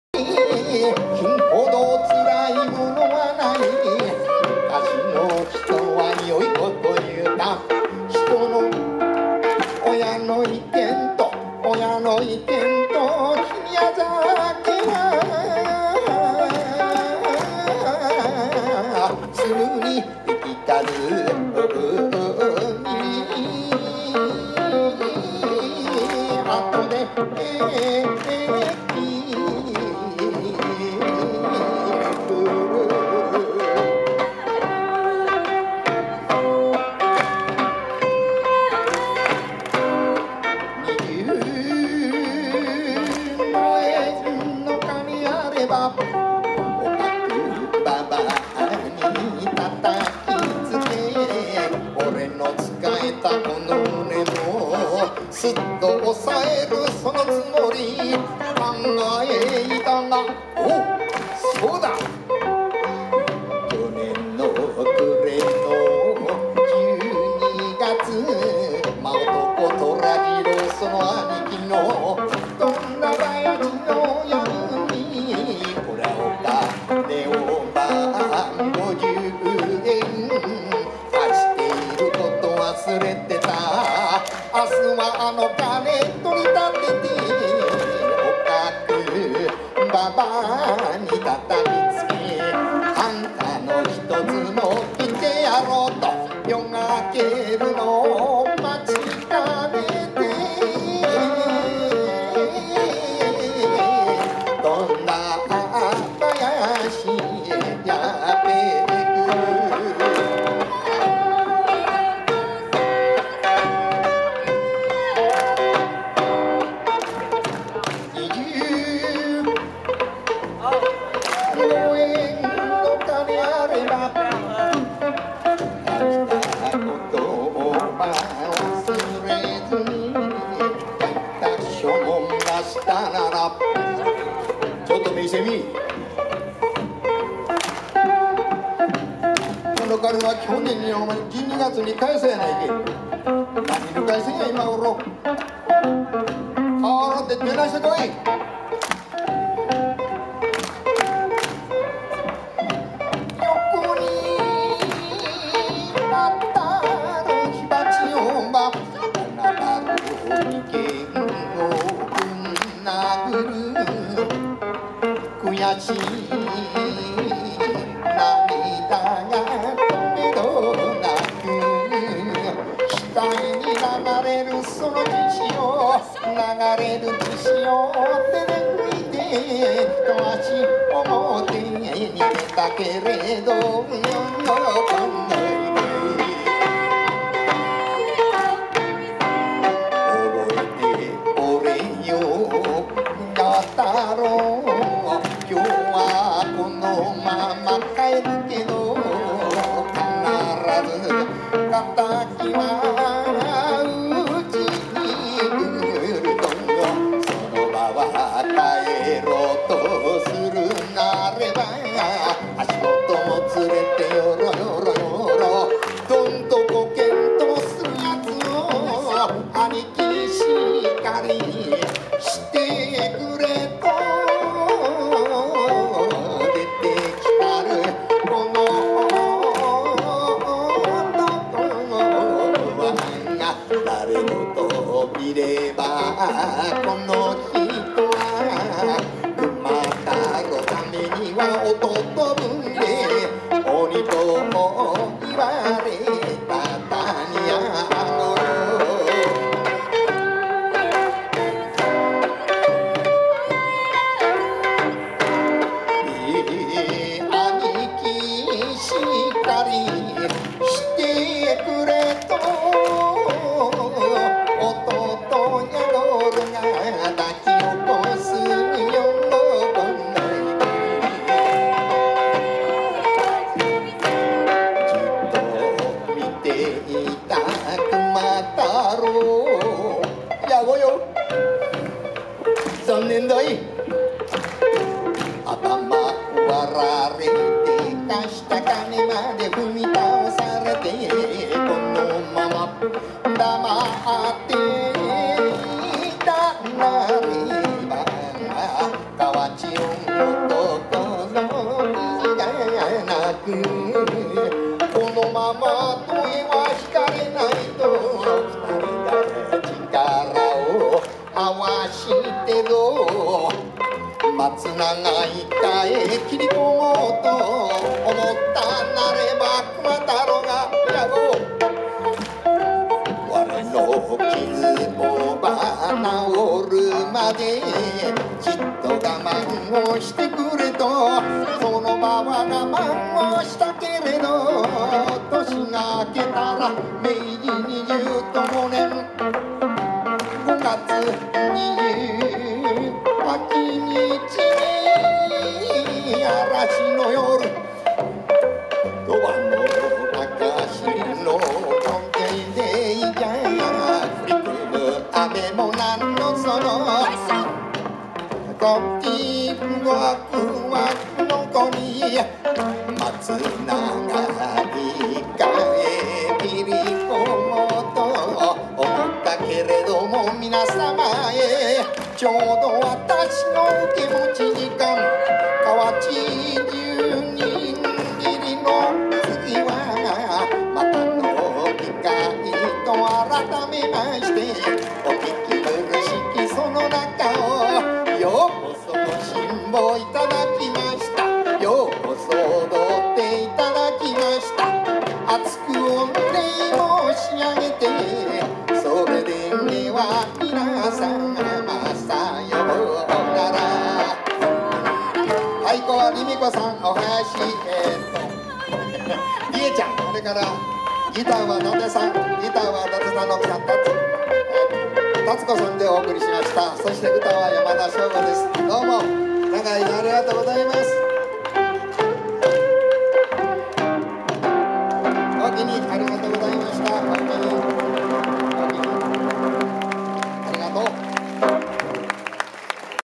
YamashoKai " Kawachi Ondo" at Osaka Matsuri
at San Francisco Japantown Peace Plaza August 27, 2016